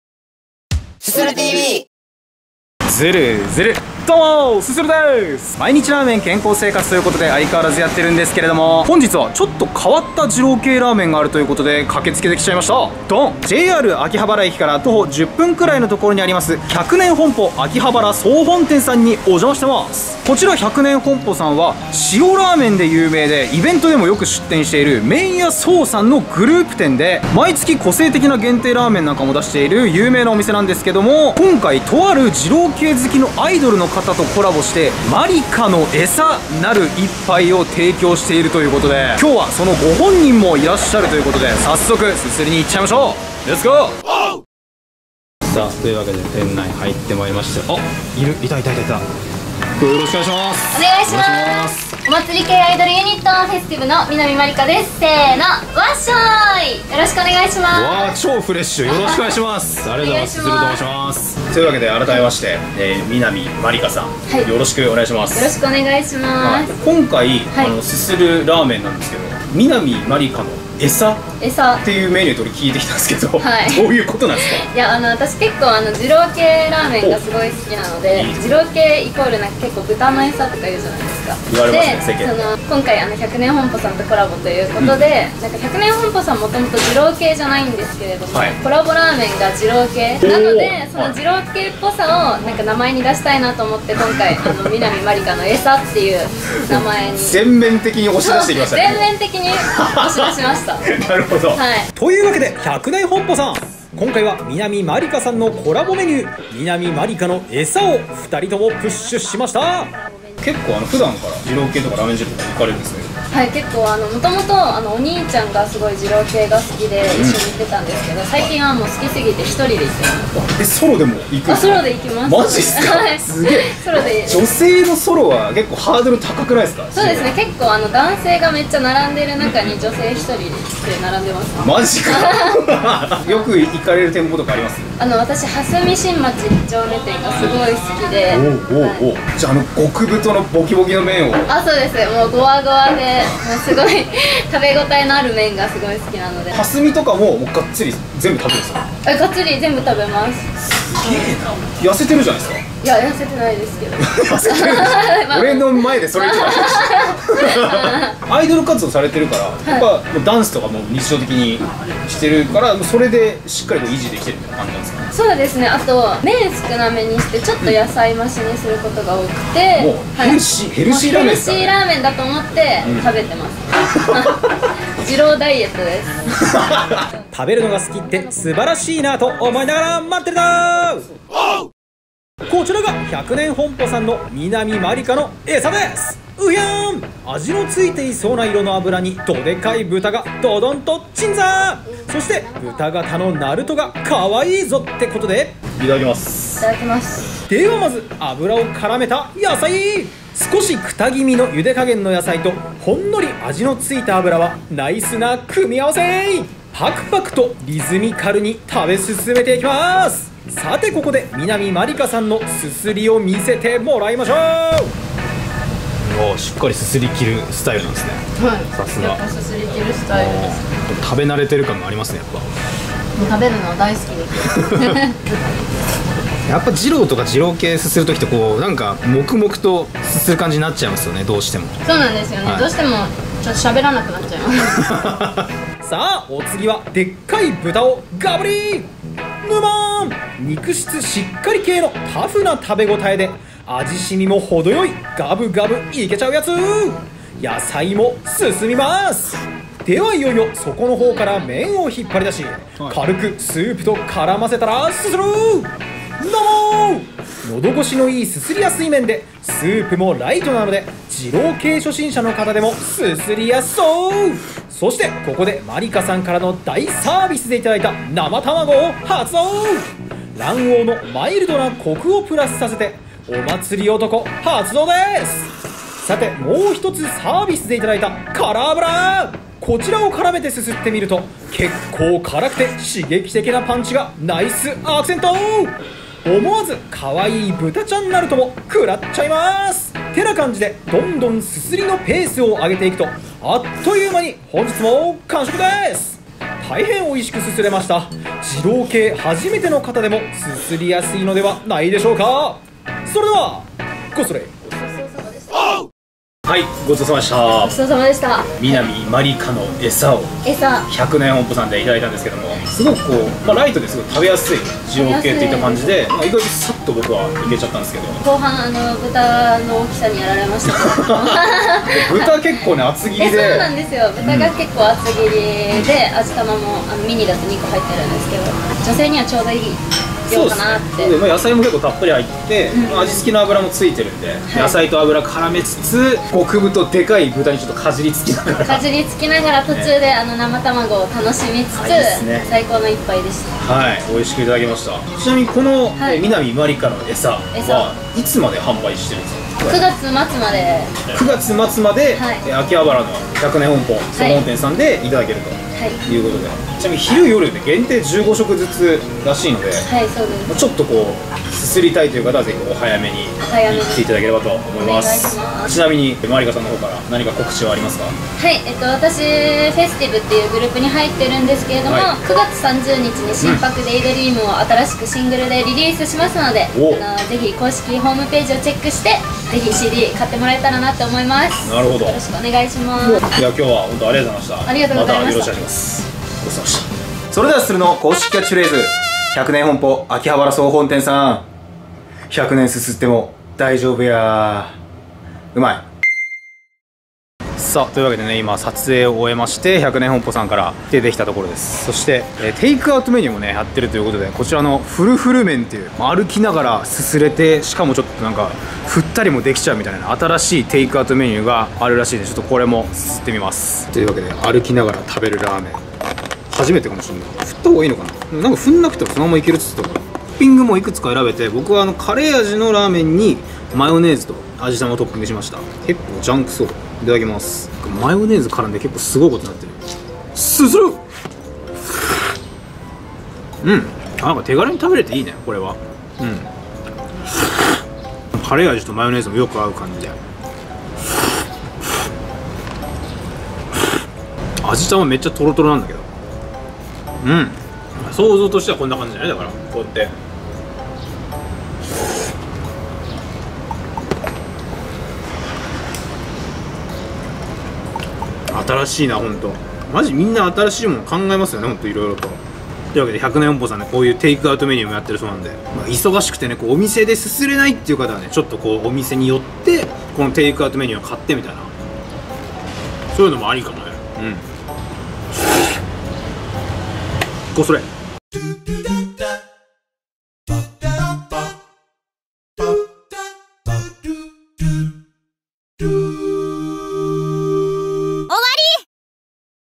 二郎系アイドル『南茉莉花』のエサがこちらです‥！をすする 百年本舗【飯テロ】SUSURU TV.第1731回
こちら百年本舗さんは塩ラーメンで有名な麺屋宗さんのグループ店！
二郎系好きアイドルの南茉莉花さんとのコラボで、
『まりかのエサ』なる一杯を提供しているとのことです。
今日はご本人と一緒にすすっていきたいと思います！
まずはヤサイとアブラを絡めてパクパクと食べ進めます。
アブラはほんのり味がついていて、クタ気味の茹で加減のヤサイと相性バッチリです。
豚は肉質しっかり系のチャーシューで、味染みも程よくこれまたヤサイが進みます！
スープは背油チャッチャ系な豚骨醤油味に近い印象で、重さも控えめですすりやすいです。
麺は喉越しの良いすすりやすい麺です。
そして辛アブラはなかなかの辛さ！刺激的な辛さでパンチがあります。
豚のイラスト書かれたナルトがかわいいなあと思いながらパクリと食らって完食しました！
食後にはフルフル麺なるテイクアウトメニューをいただきました。
こちらはカレーとマヨネーズでジャンクな味を楽しめました！

【本日のラーメン情報】
百年本舗 秋葉原総本店
東京都千代田区外神田3-8-9 昌徳ビル1Ｆ

もしくはスタンプショップで「susuru」と検索してください！

〒164-0001
東京都中野区中野3-42-2
SUSURU宛　まで！